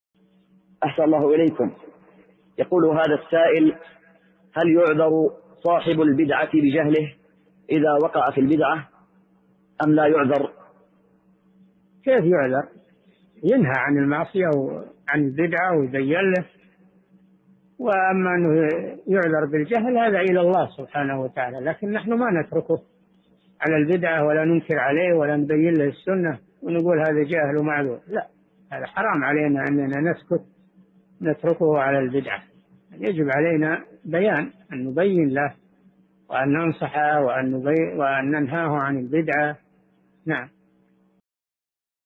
[111 /186] هل يعذر صاحب البدعة بجهله إذا وقع في البدعة أم لا يعذر؟ الشيخ صالح الفوزان
[111 /186] هل يعذر صاحب البدعة بجهله إذا وقع في البدعة أم لا يعذر؟
 الجواب: كيف يعذر؟ ينهى عن المعصية وعن البدعة ويبين له وأما أنه يعذر بالجهل فهذا إلى الله سبحانه وتعالى، لكن نحن ما نتركه على البدعة ولا ننكر عليه ولا نبين له السنة ونقول هذا جاهل ومعذور! لا، هذا حرام علينا أننا نسكت ونتركه على البدعة بل يجب علينا البيان أن نبين له وأن ننصحه وأن ننهاه عن البدعة.
السيرة الذاتية :
للاشتراك في قناة الشيخ صالح الفوزان

للاستفسار والإقتراحات والدعم
رئيس الفريق
لمشروع كبار العلماء